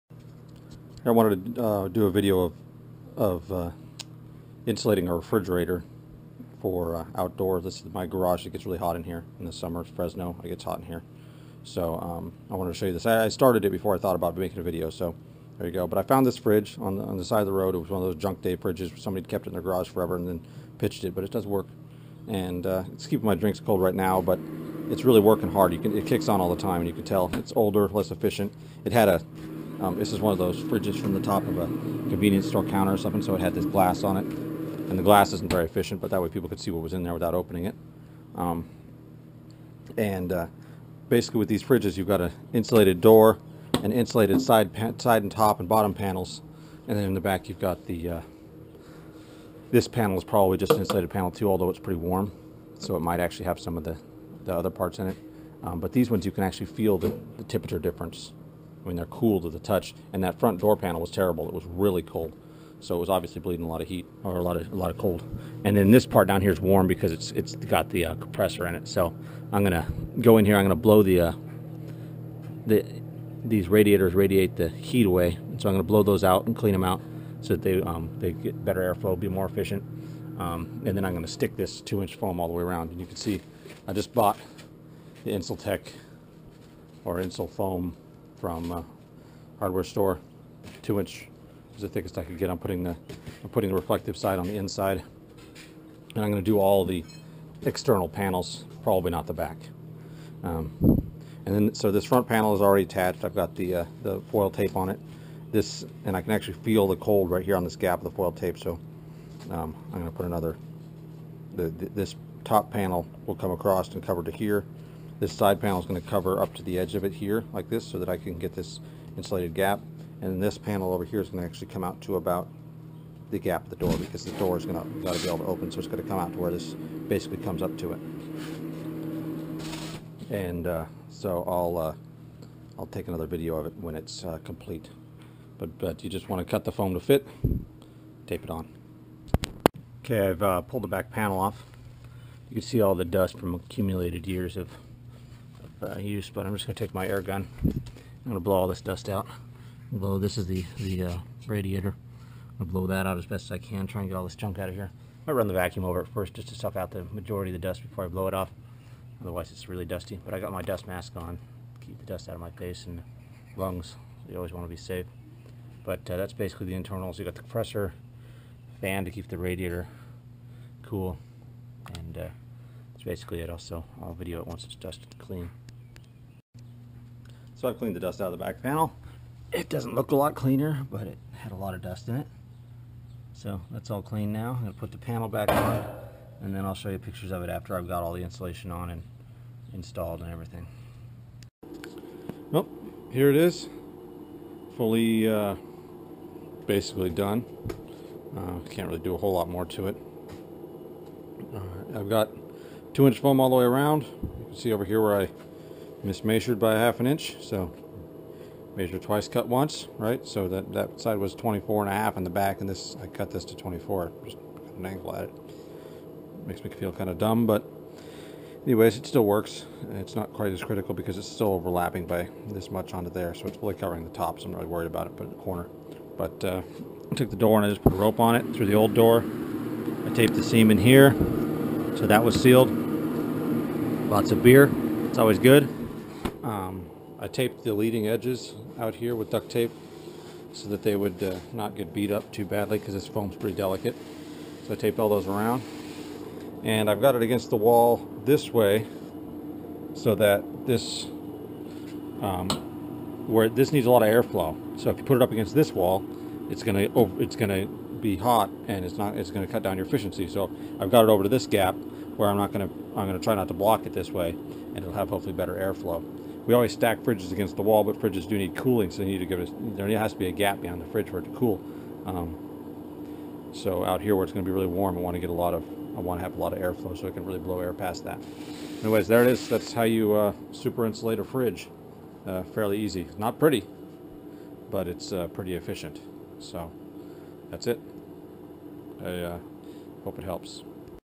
How to insulate a refrigerator
This video shows how to insulate a refrigerator. For better efficiency.

GoPro Hero 5 black
GoPro Sesson
GoPro Microphone adapter
GoPro Case
Wasabi power batteries for GoPro